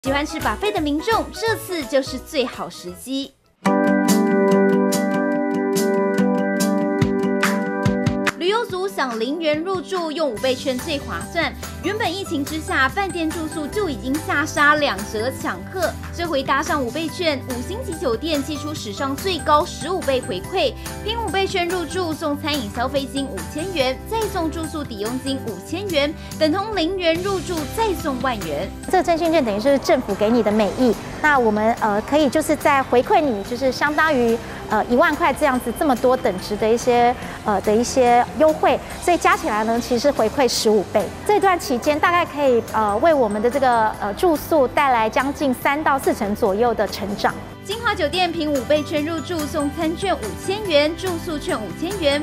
【新聞】五倍券怎麼用最划算？０元帶你入住五星酒店！｜非凡財經新聞
數位晶華，將帶給你全新的影音世界！欲看更多精彩內容，請上